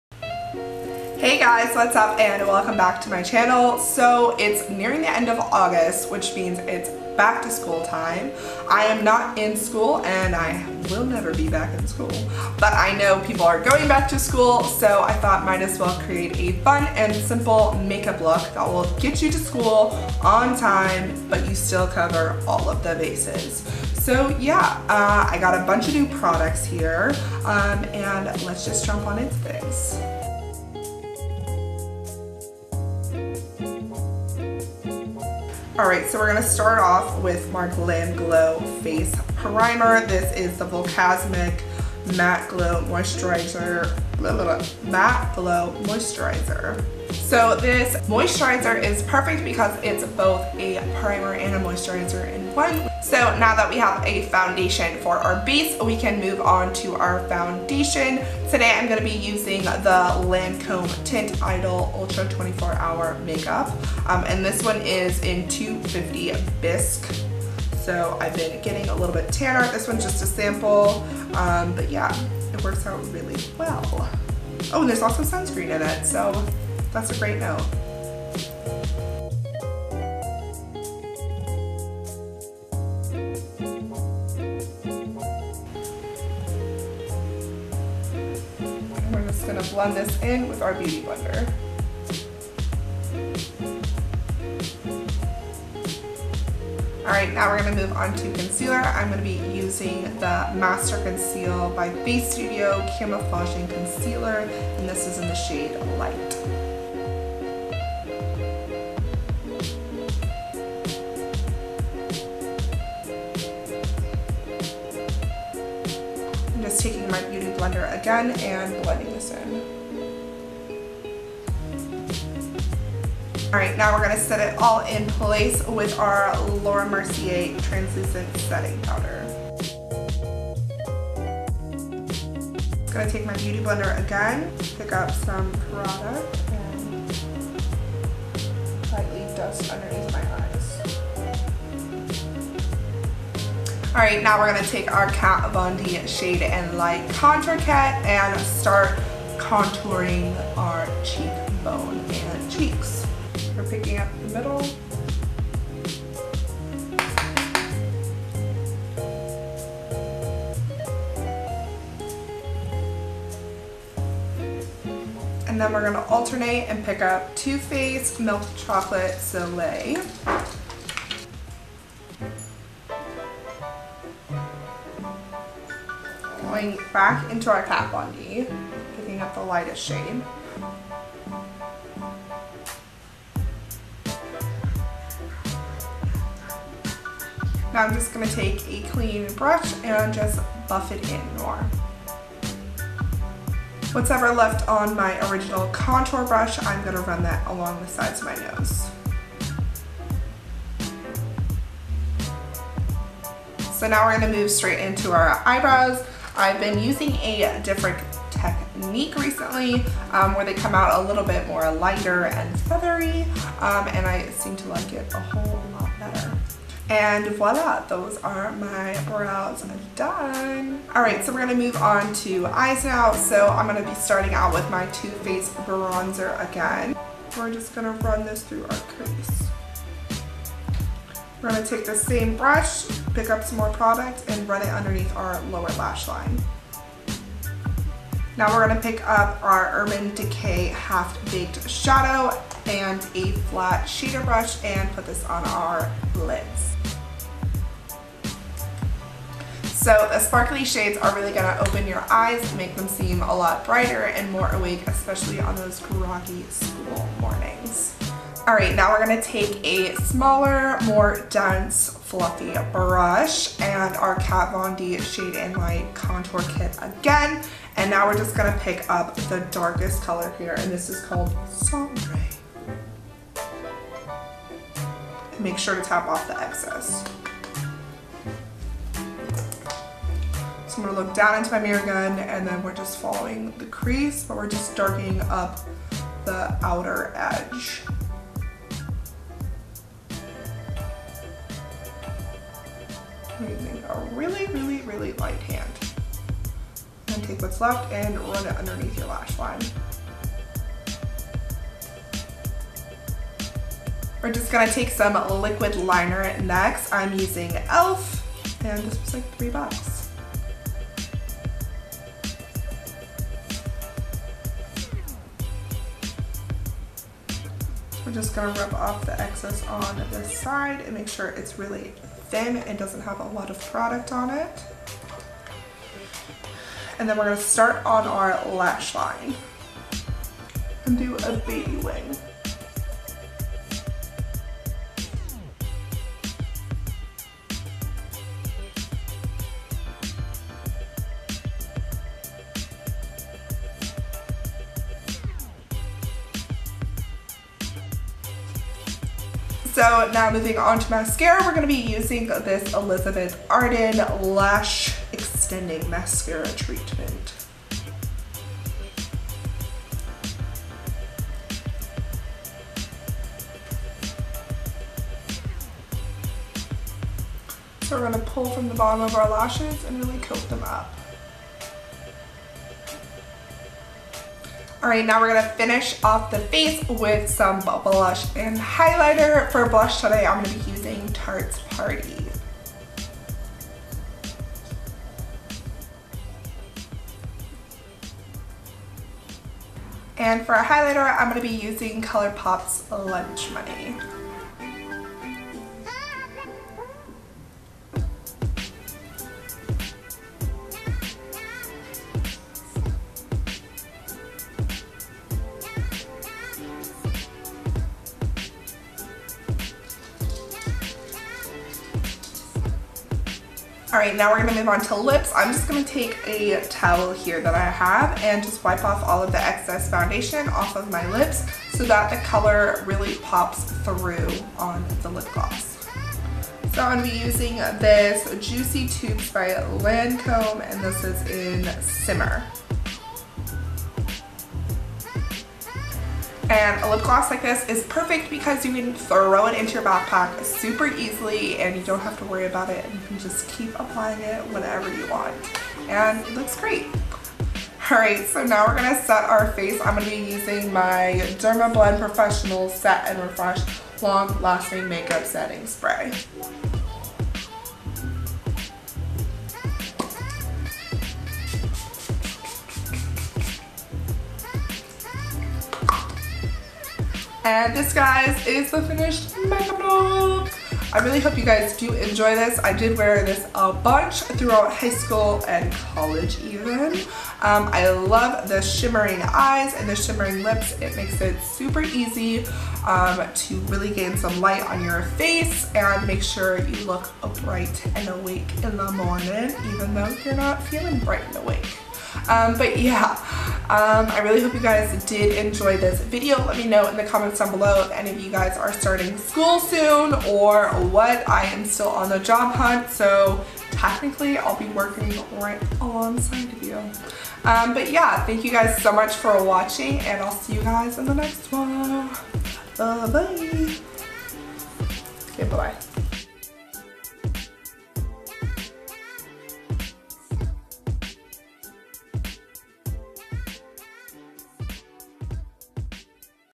Easy Back To School Makeup
This back to school makeup tutorial is the perfect first day of school outfit. This look is a super easy to makeup and gets you out of the door fast! Not to metion these shimmering eyes and lips will bring even more light to your face making you look more awake.

Products used:
GlamGlow Volcasmic Moisturizer
Lancome Tint Idole Foundation, 250
Maybelline Camouflaging Concealer, light
Laura Mercier Translucent powder
Beauty Blender
Kat Von D Shade & Light  Contour Kit
Too Faced Milk Chocolate Soleil Bronzer
Anastasia Beverly Hills Dip Brow Promade, Soft Brown
PUR cosmetics eyebrow pencil, latte
Tarte Paarrty Blush
ColourPop Highlighter , Lunch Money
Urban Decay Eyeshadow, Half Baked
ELF liquid liner
Elizabeth Arden  mascara
Estee Lauder Lipgloss , shimmer